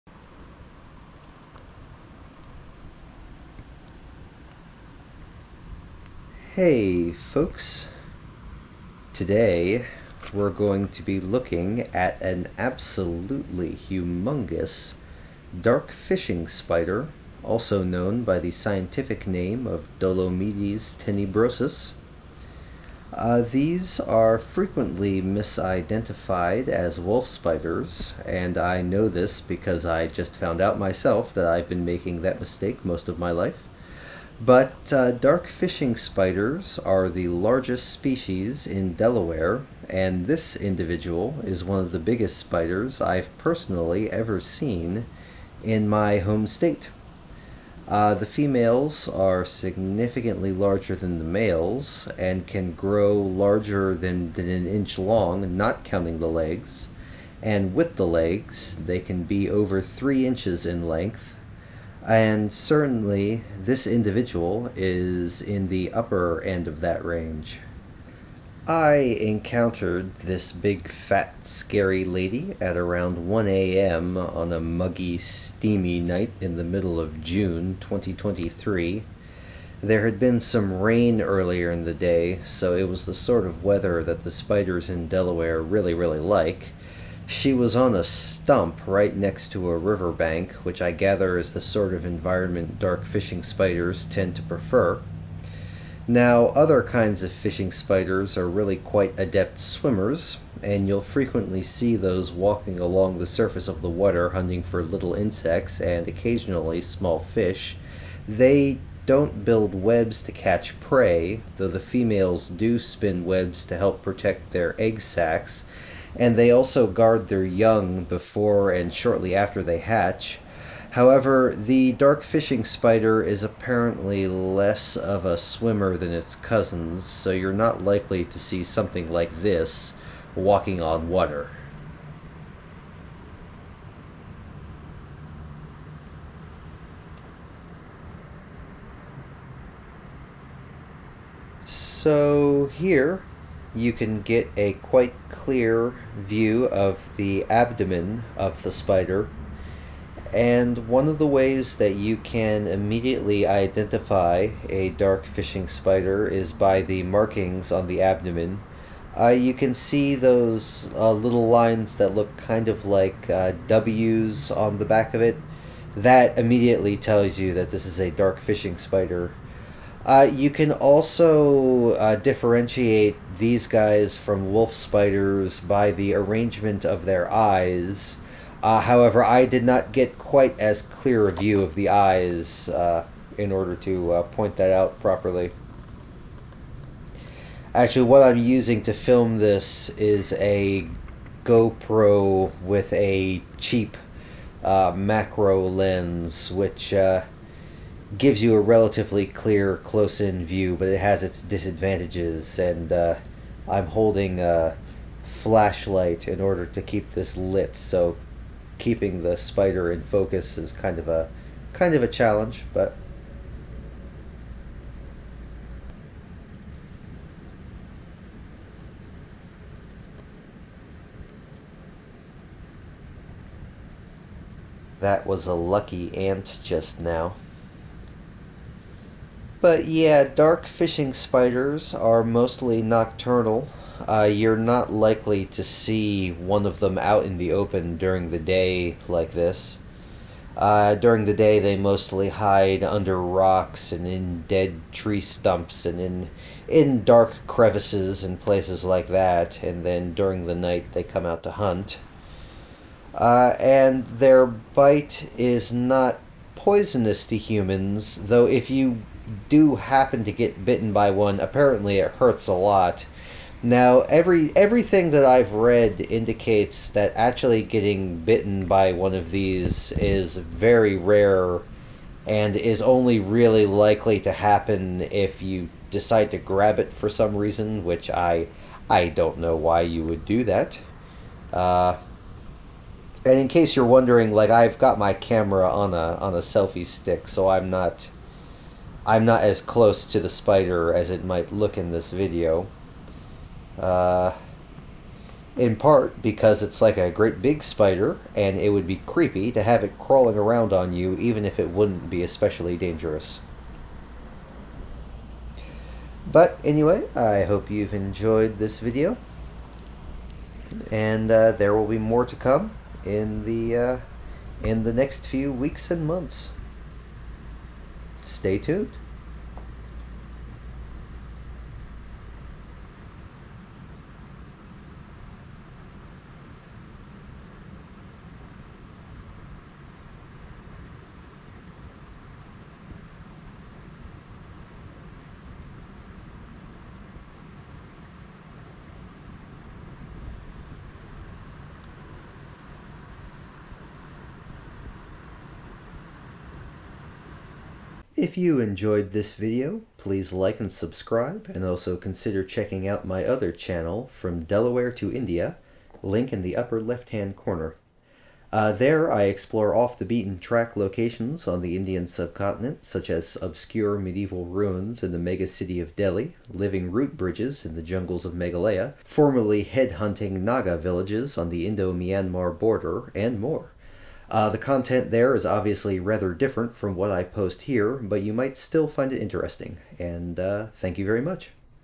The most monstrous spider in Delaware
The dark fishing spider, or Dolomedes Tenebrosus, is the largest native spider found in the state of Delaware. Females can have bodies that are an inch long, not including their legs. With their legs, dark fishing spiders can be over three inches across. The spiders are instantly recognizable by the large “w” markings on their abdomens.

Their preferred habitat is near the edges of ponds and streams. Other, less massive varieties of fishing spider are able to walk across the surface of slow-moving bodies of water as they hunt for insects and small fish (hence the name “fishing spider”), however this behavior is less common in Dolomedes Tenebrosus. They are also nocturnal, hunting their prey rather than trapping it with webs. The females are still capable of producing silk, but they mostly use it to hang their egg sacks on leaves and branches. Femail dark fishing spiders are also known to protect their egg sacks, and then their young immediately after they hatch.

While it has been reported that dark fishing spiders can deliver a painful bite to humans— large individuals like the one in this video certainly look capable of inflicting a serious wound — such instances are very rare. The species usually prefers to escape if confronted with a human and is only likely to bite if the person stresses it out by deliberately grabbing it.